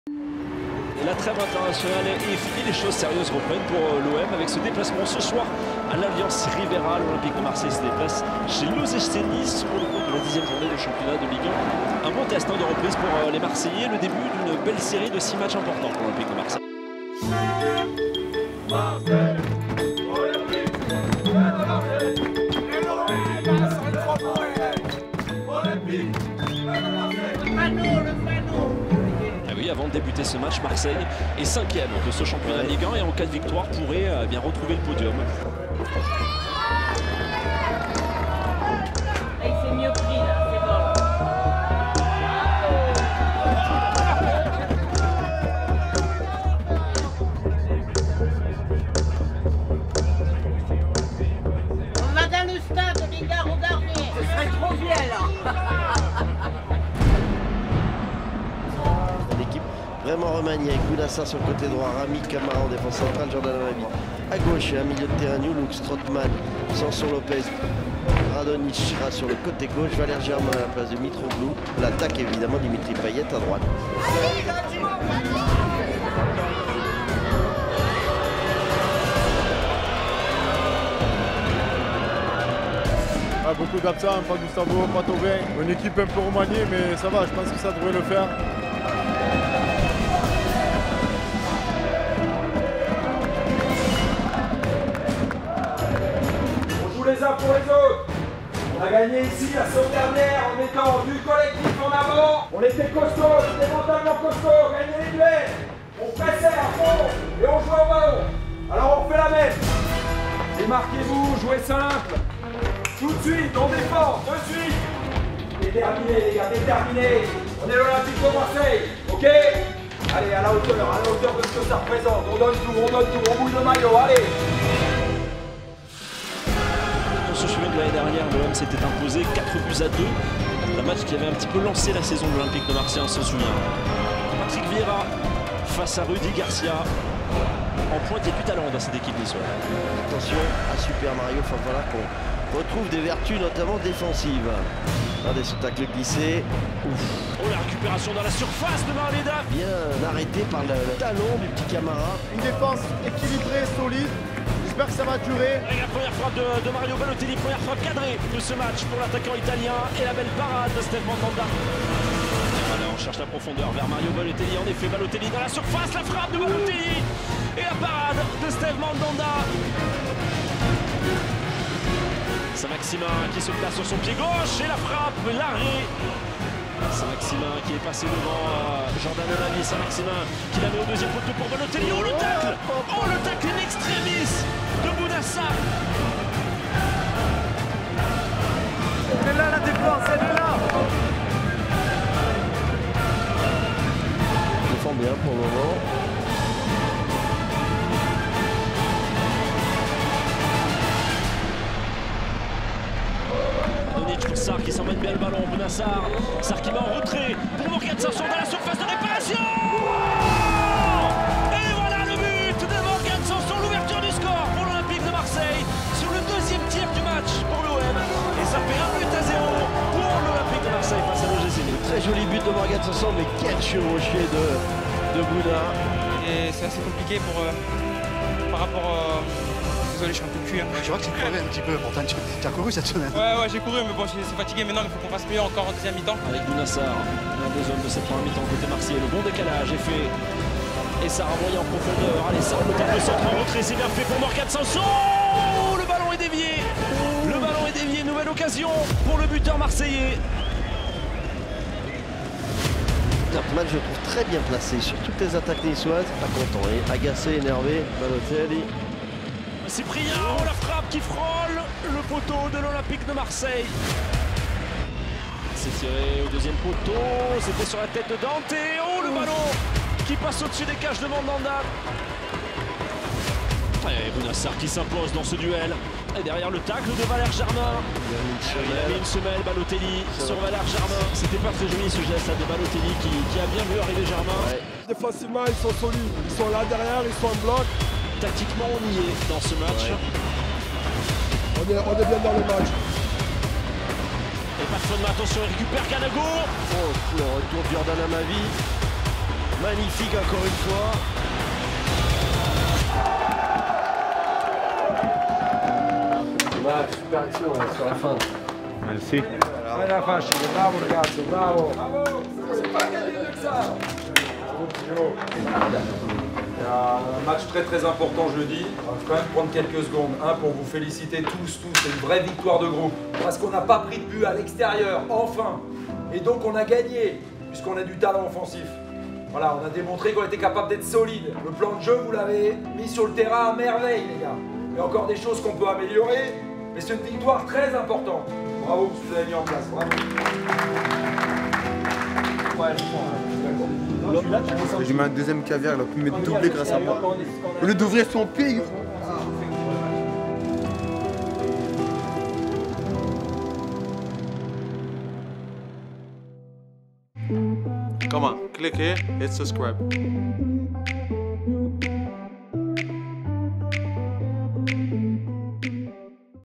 Nice 0-1 OM, Aftermovie de la rencontre | JOUR DE MATCH
JOUR DE MATCH saison 1 "EN BLOC"
La victoire à l'ALLIANZ RIVIERA depuis l'intérieur, revivez ce derby méditerranéen depuis l'intérieur, découvrez le discours du coach de l'OM après la rencontre ...